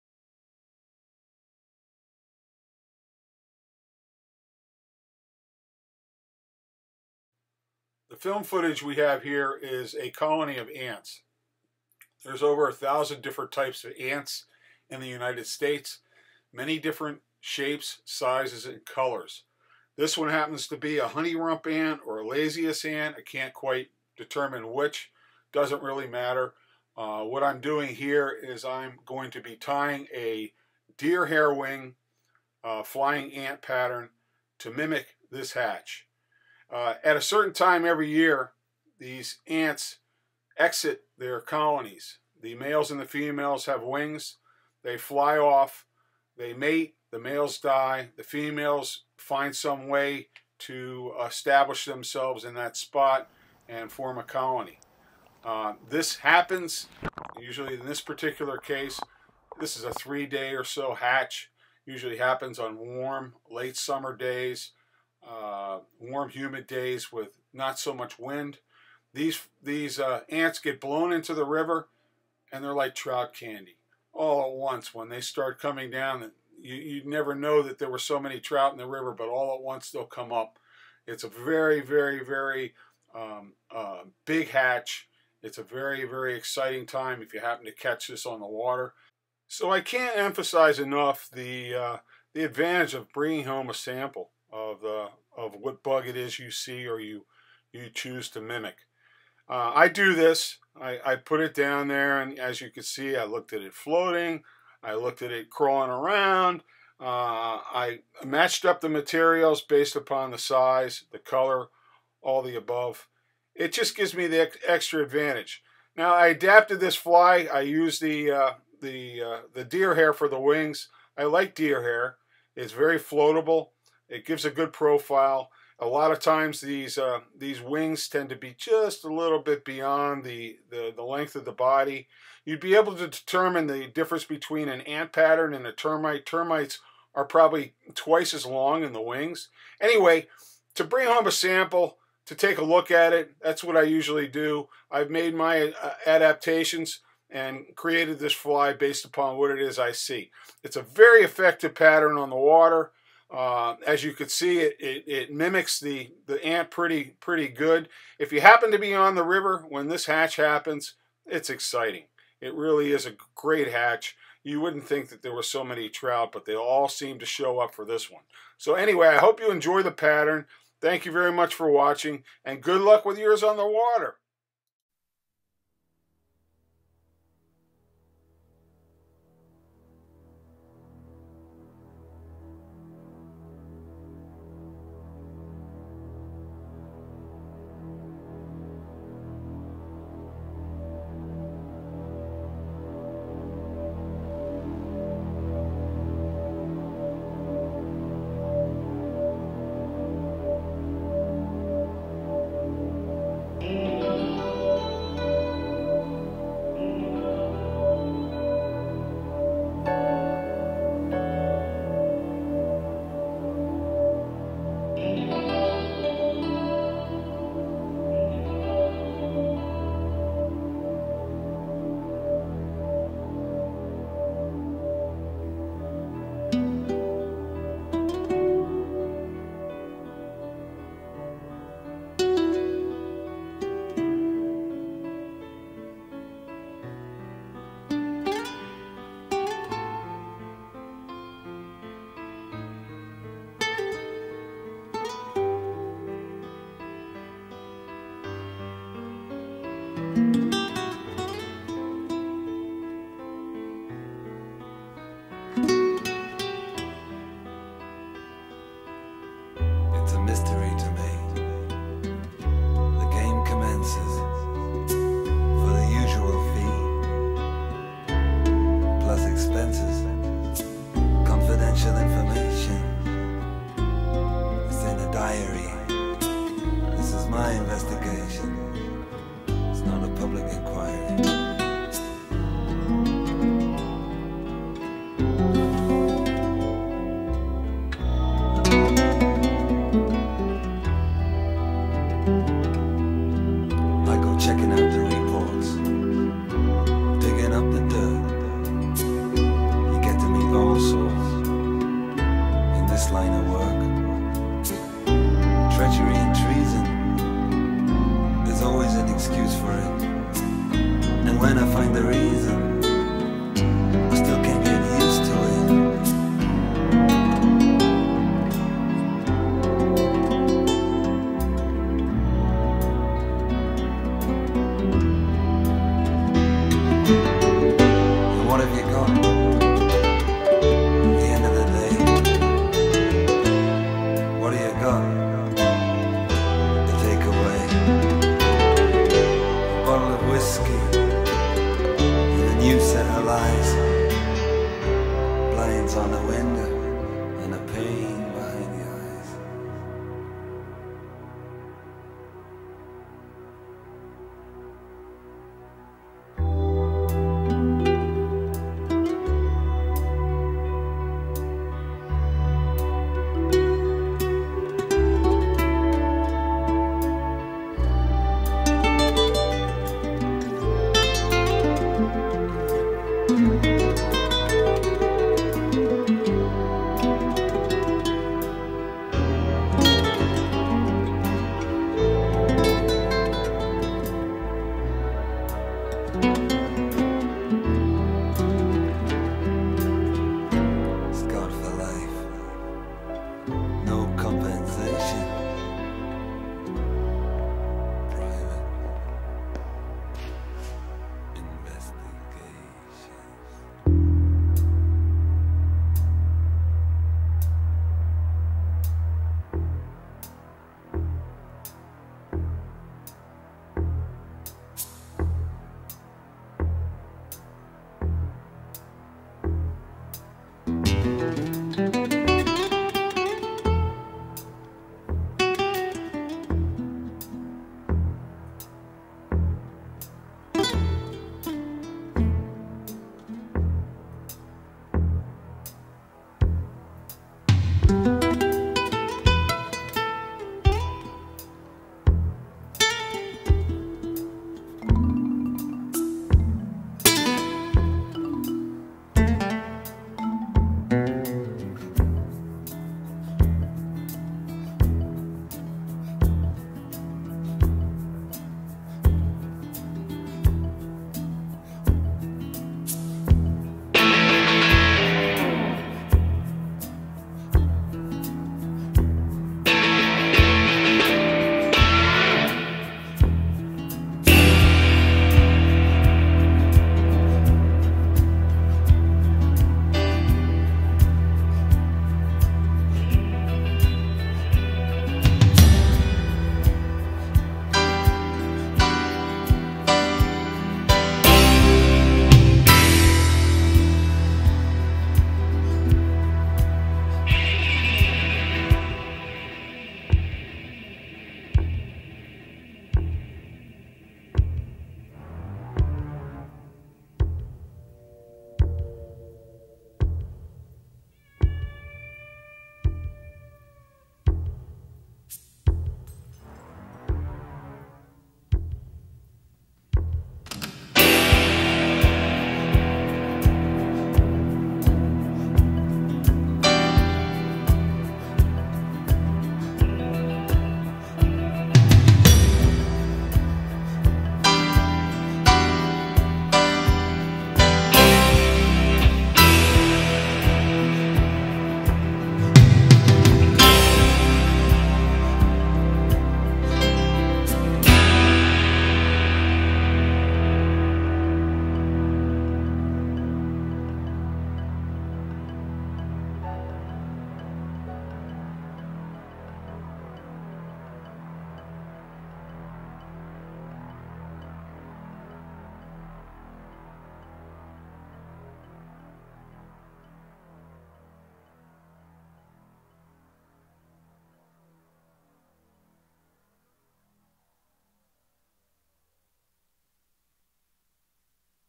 FLY TYING- "HONEYBUTT" HAIRWING FLYING ANT-THE DEADLIEST FLIES with Chris Walklet
This video is about FLY TYING- HONEYBUTT HAIRWING FLYING ANT. Each year torwards the end of Summer into early Fall if you are fortunate enough to be on the water for the flying ant hatch and have the pattern to match the hatch it can be as good as any hatch all season. In many cases you will get to see all the trout that are in your stream including the reluctant lazy big ones feast on the plum flying ants as the float helplessly along. There are many types of ants in the world so tuning into what is on your stream is a big benefit. Otherwise to have ants in different colors from in this case ginger down to darker shades and black is a good idea. On this day there were 3 hatches going on at the same time. The first was a small ant too small to tie in black, The second was a black flying ant size 20 and the third was this honey rump ant. It was an exciting day if you enjoy topwater fly fishing. This fly is tied on a size 16 Partridge light dry fly hook, Ginger fine dubbing, Rusty rooster hackle, Uni 8/0 tan thread, and coastal deer hair as my final adaptation for this pattern. Good luck with yours.